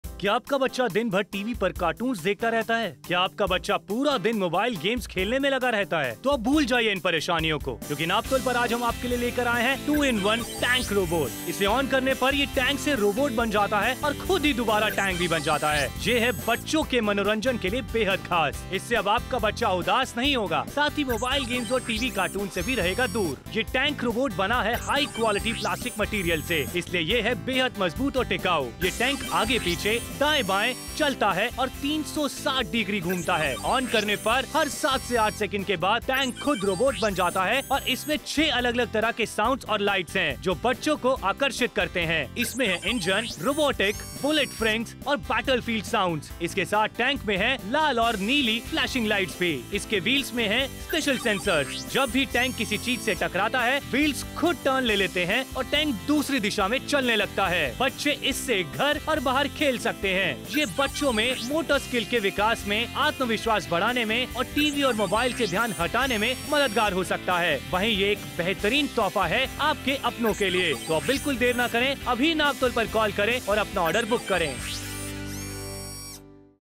Robo Tank Toy -LPO Theme (Code:9092) SHORT VIDEO 21401
Robo Tank Toy Rs.699 -LPO Theme (Code:9092)

Enter into a world of excitement and fun with this amazing Robot Tank Toy that transforms from Tank to robot and back to the Tank. Playing with this toy enhances the imagination and creativity of the child. The sound effect and blinking lights help your kid to develop their sensory organs. The sturdy and durable designs do not have any sharp edges and are safe for your kid to play. The high-quality ABS plastic material of the toy is lightweight, non-toxic & eco-friendly. Robot tank Toy is easy to operate and will leave you children mesmerized and entertained all the times.

Highlights:
2 in 1 Toy: Transforms from a robot to Tank toy
Functions: Moves Forward-Backward, Right-Left
Rotates 360 Degree
Bump & Go: The wheels have a special sensor. When the Tank drives and collides with something, the wheels will automatically turn, and the tank can continue to drive well
Comes with music and light effect
Operate without remote control & single on/off switch
Portable, lightweight & easy operation
Kids can play indoor and outdoor
Develops eye-hand coordination
Improves motor skills
Sense obstacle
Changes direction automatically
Ideal for gifting

Features:
Material: Plastic
Age Group: 3 years+
Ideal for: Boys / Girls
Powered by: 3 AA (Not included in the box)
Functions: Moves Forward, Backward, Right, Left, Rotates 360 degree
Transformation Time: 7-8 Sec
Different Sounds: Realistic engine and acceleration, Robotic, Bullet Firings, Battle field sounds
Light: Red, green & blue flashing lights
Dimensions: 22.5 x 10.2 x 9.7 cm
Transfomer Robot Height: 20.6 cm
Weight: 372 g